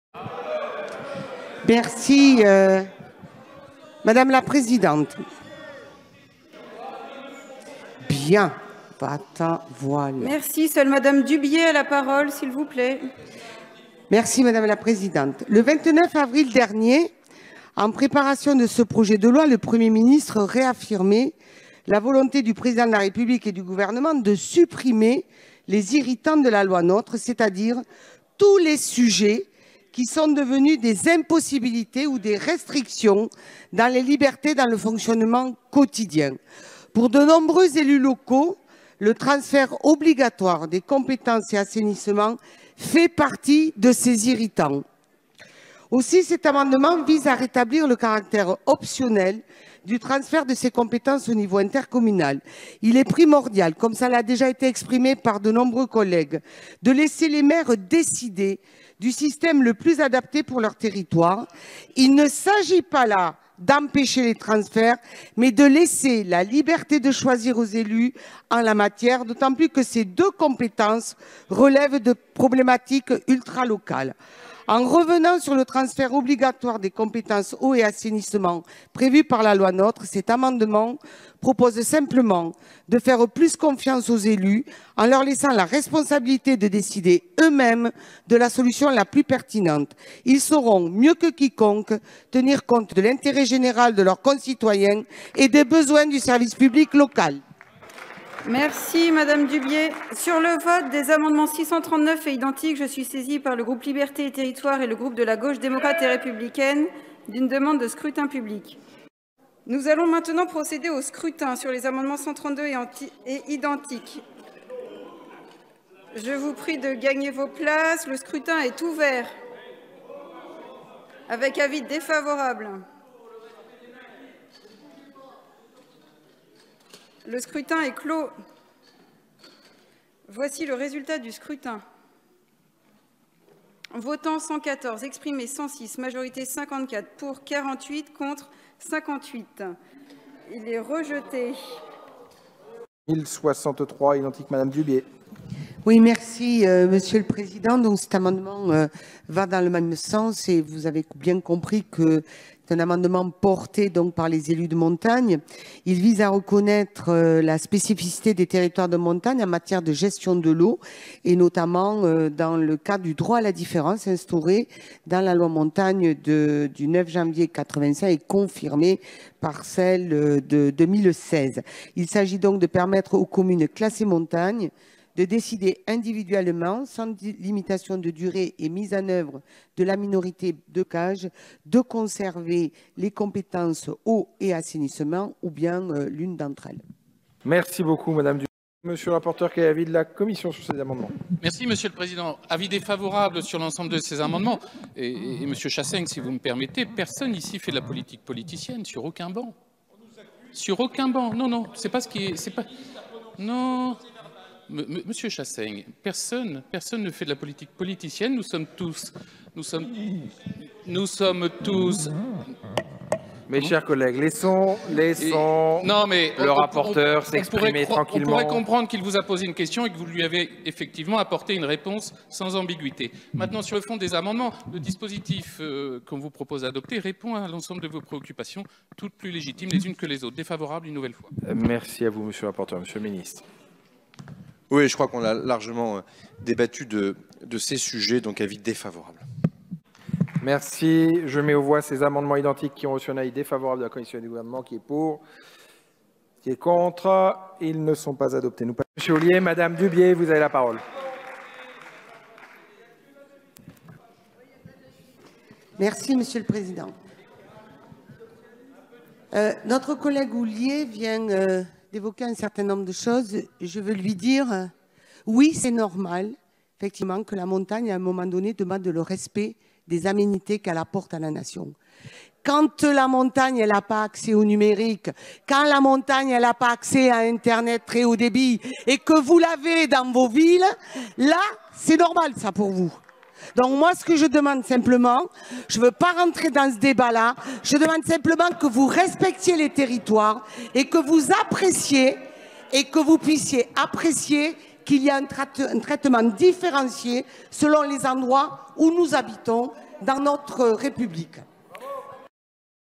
Transfert de compétence Eau et Assainissement vers les intercommunalités
[ Engagement & Proximité - Eau & Assainissement ]

La semaine dernière l'Assemblée Nationale a examiné le projet de loi "Engagement & Proximité".

L'article 5 prévoit le transfert obligatoire des compétences "eau" et "assainissement" au niveau intercommunal au 1er janvier 2026, à l'exception des communes membres de communautés de communes qui n'exercent pas ces compétences aujourd'hui et qui auront exprimé leur opposition au transfert avant le 1er janvier 2020.

Mardi 19 novembre, lors de débats houleux dans l'hémicycle, j'ai défendu un amendement visant à rétablir le caractère optionnel de ce transfert de compétences - comme l'avait adopté le Sénat. Malheureusement, l'Assemblée Nationale n'a pas adopté cette proposition.

Suite à ce rejet, j'ai défendu d'autres amendements visant à modifier cet article 5. J'ai notamment appelé à prendre en compte la spécificité des territoires de montagne en permettant aux communes classées montagne de décider individuellement et sans limitation de durée de conserver toute ou partie des compétences « eau » et « assainissement ». Toutefois, aucun de mes amendements n'a été adopté par l'Assemblée Nationale.